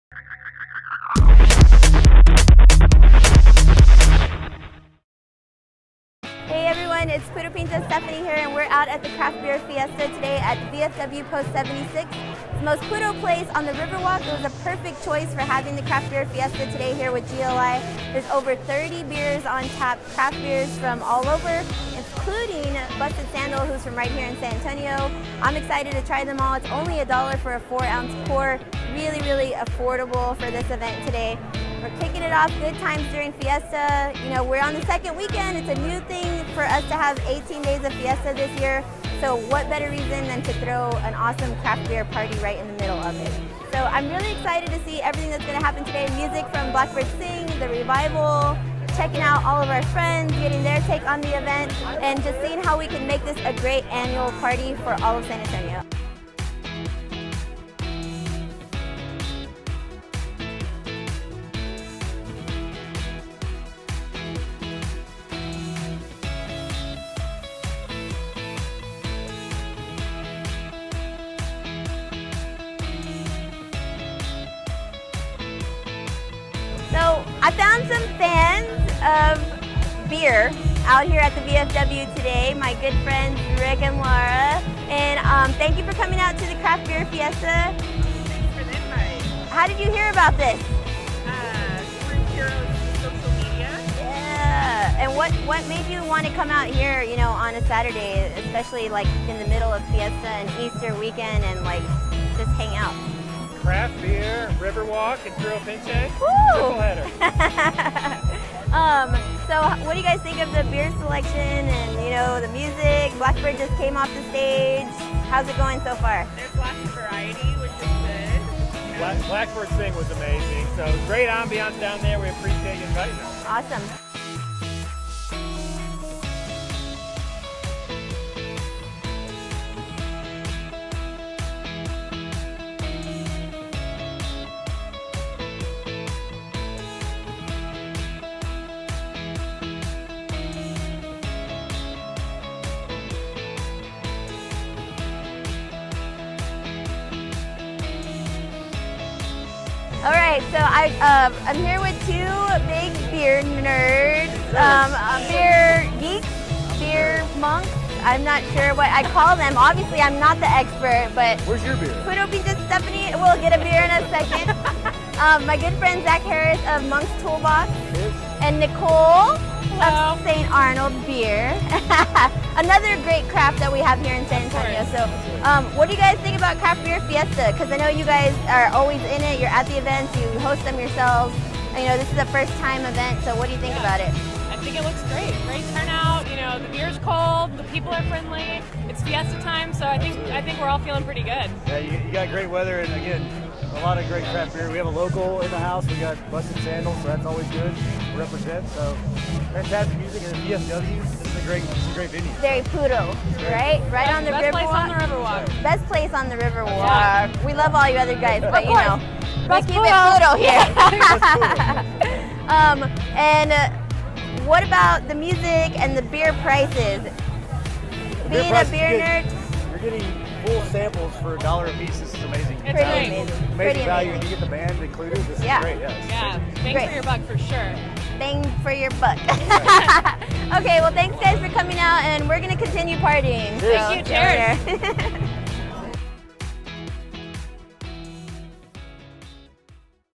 Craft Beer Fiesta 2014
Puro Pinche Presents the Craft Beer Fiesta 2014 at the VFW Post 76 in Downtown San Antonio.